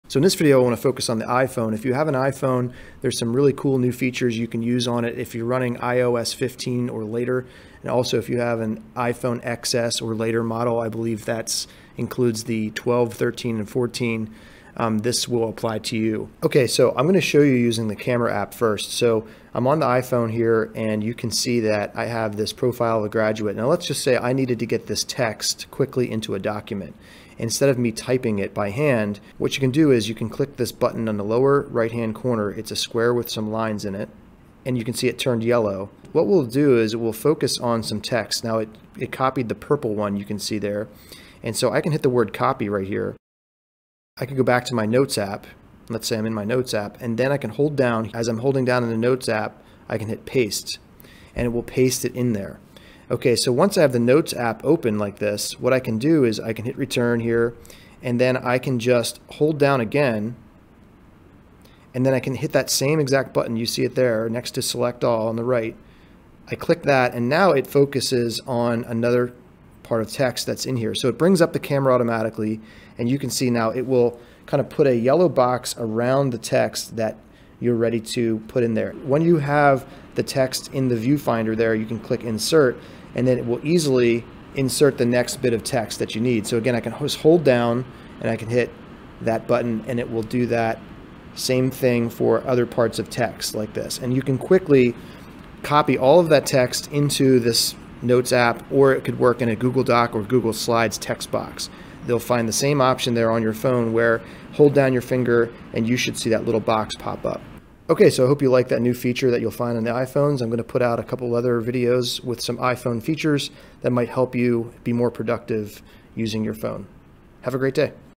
New iPhone Text Recognition Feature! No Typing!
How to capture text using your iPhone